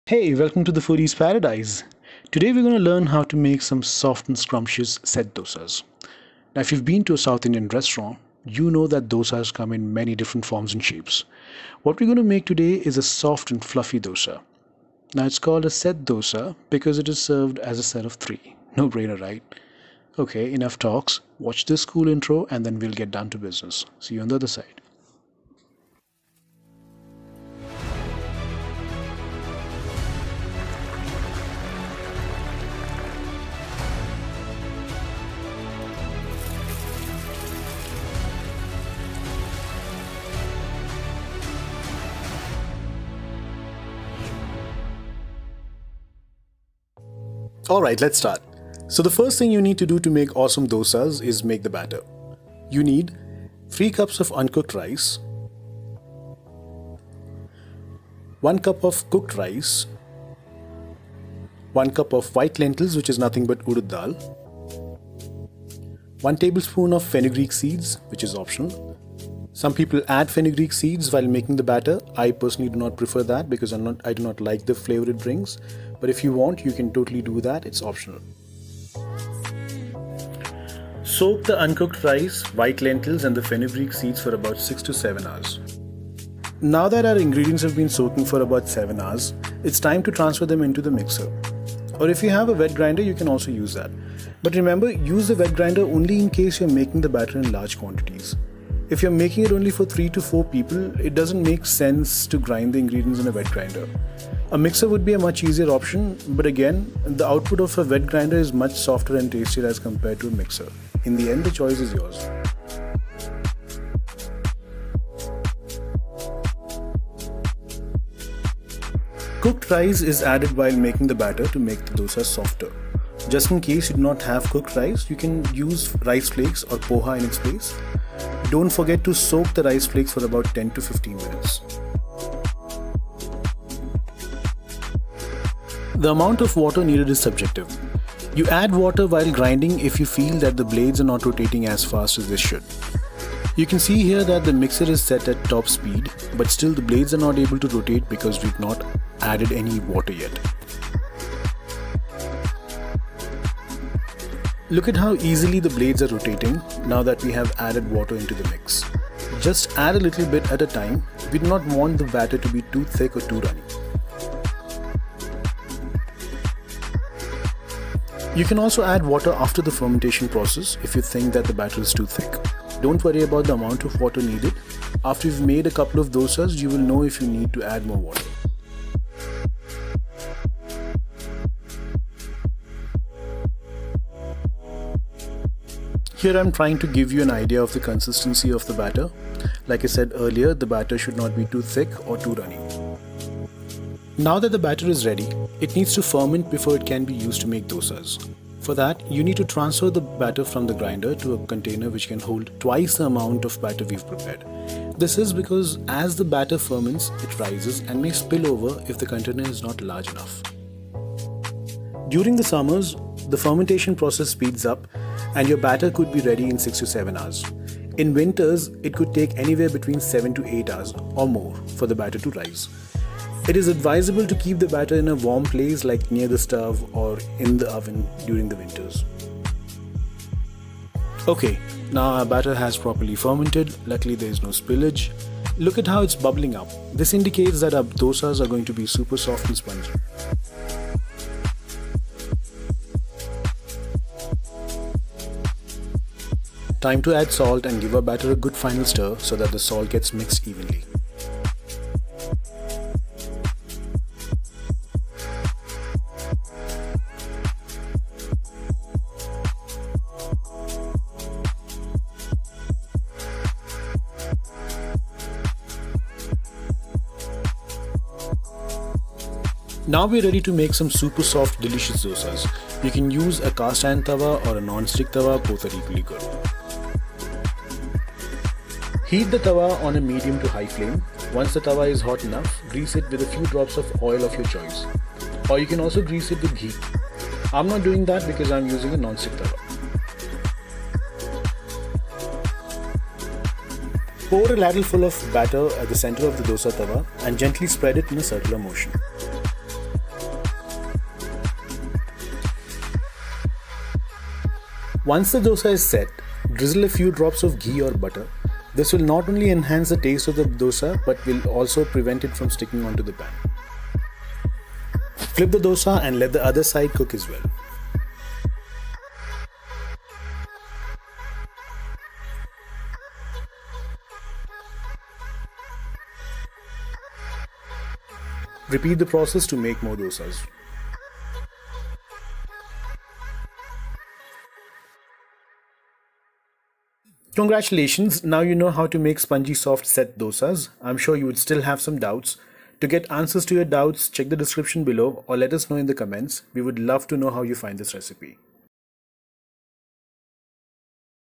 How to make Dosa
Detailed description of how to make set dosas. The same batter could be used to make idlies.

Ingredients

Uncooked rice - 3 cups
Cooked rice - 1 cup
Udad dal - 1 cup
Fenugreek seeds - 1 tbsp (optional)

HOW TO MAKE THE BATTER

1. Take the uncooked rice, udad dal and Fenugreek seeds and wash them thoroughly. 2. Soak them in fresh water for about 6 - 7 hours.
3. Once the ingredients are soaked well, transfer them in a mixer bowl. Add the cooked rice into the mix.
4. Add water as required.
5. Grind them until it reaches desired consistency.
6. Transfer the batter into a container that can hold twice the volume of the batter you prepared.
7. Close the lid and leave it to fermant for 7 - 8 hours.

1. Heat a dosa tawa on a medium to high flame.
2. Grease the pan with oil or ghee.
3. Pour a laddle full of batter at the center of the tawa and spread it in a circular motion.
4. Once the dosa is set, drizzle some ghee.
5. Flip the dosa and let the other side cook too.
6. Serve it with your favourite chutni or sambhar

Please share your feedback. Let us know if you have any doubts. Also let us know what other recipe you would want us to upload.